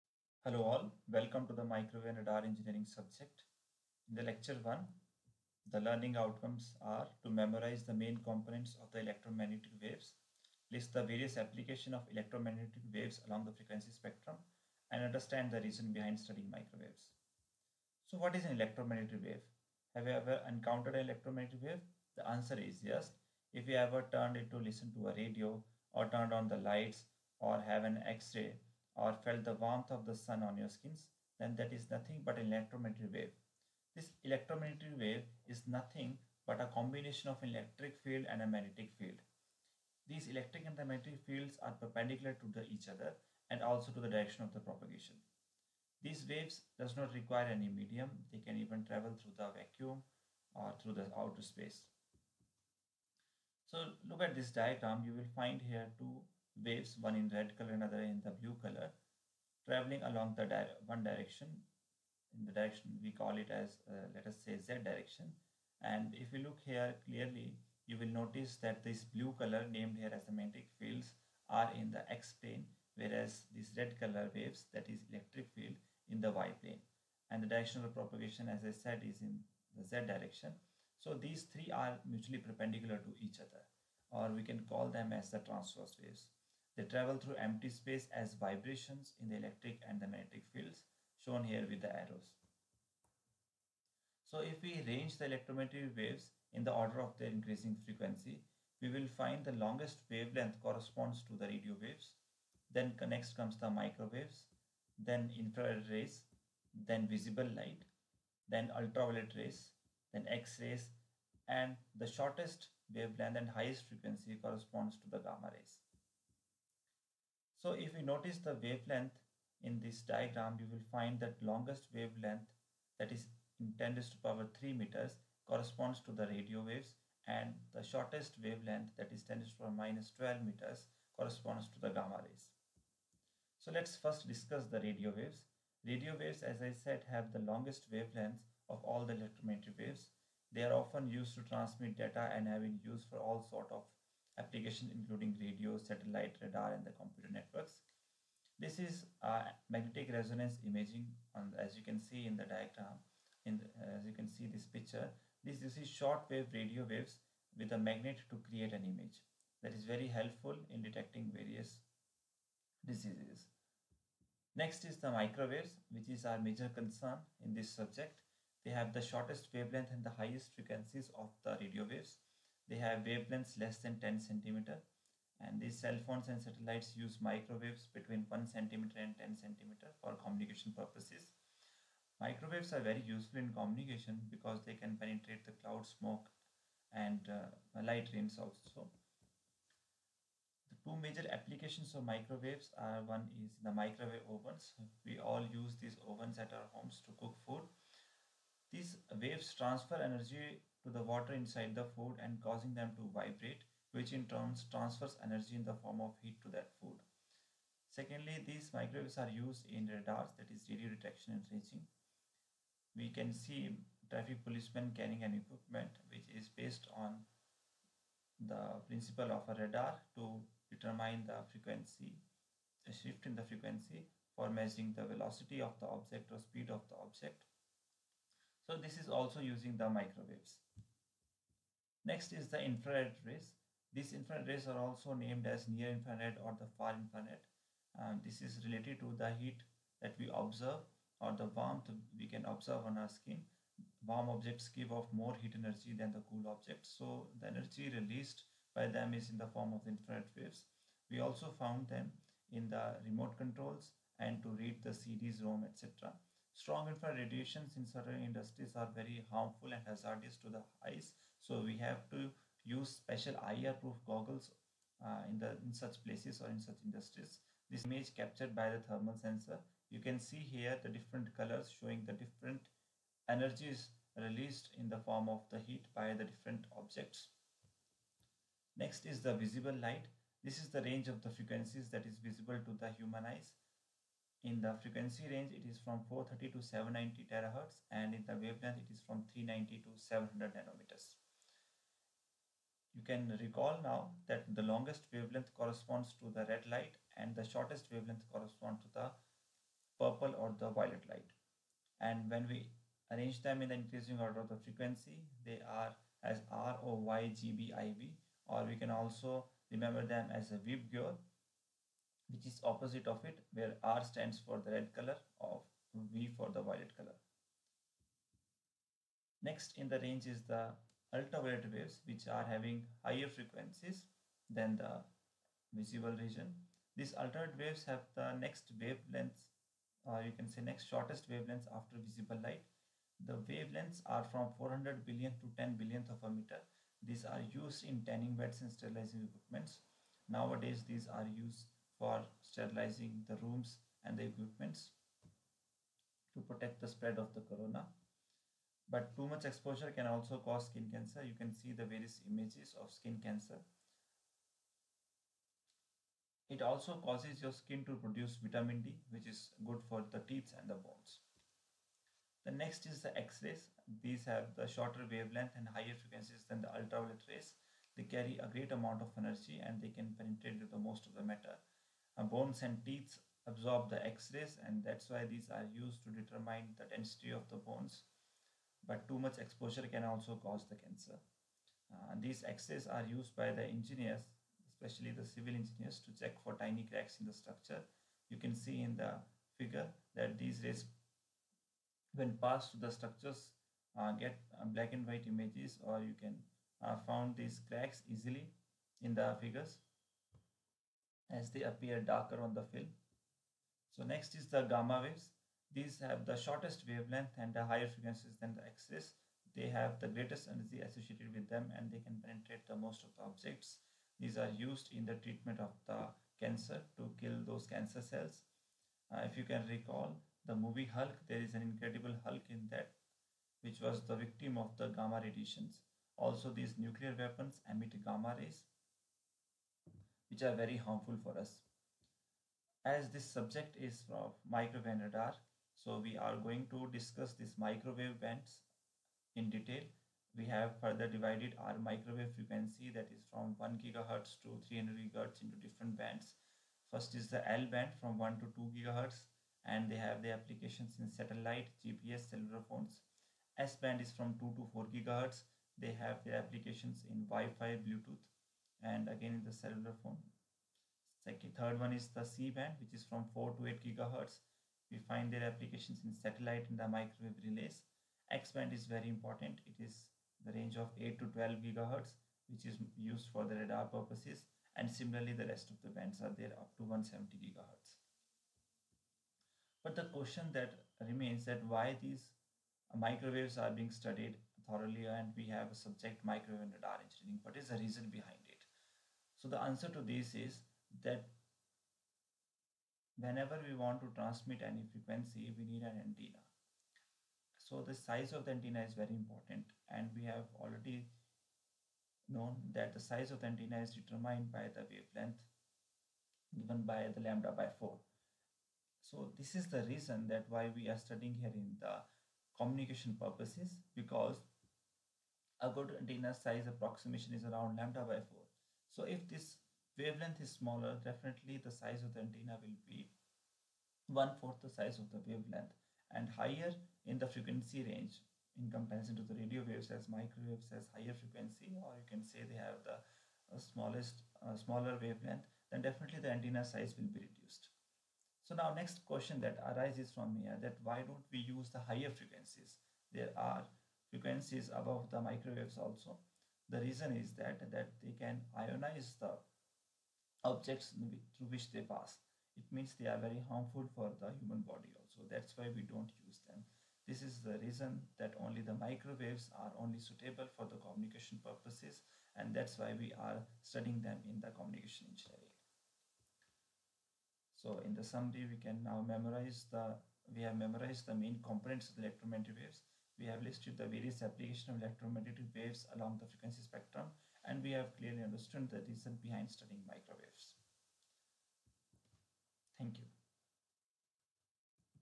Electromagnetic Spectrum (Microwave and Radar Engg. Lecture 1)
The electromagnetic spectrum is the range of frequencies (the spectrum) of electromagnetic radiation and their respective wavelengths and photon energies.

The electromagnetic spectrum covers electromagnetic waves with frequencies ranging from below one hertz to above 1025 hertz, corresponding to wavelengths from thousands of kilometers down to a fraction of the size of an atomic nucleus. This frequency range is divided into separate bands, and the electromagnetic waves within each frequency band are called by different names; beginning at the low frequency (long wavelength) end of the spectrum these are: radio waves, microwaves, infrared, visible light, ultraviolet, X-rays, and gamma rays at the high-frequency (short wavelength) end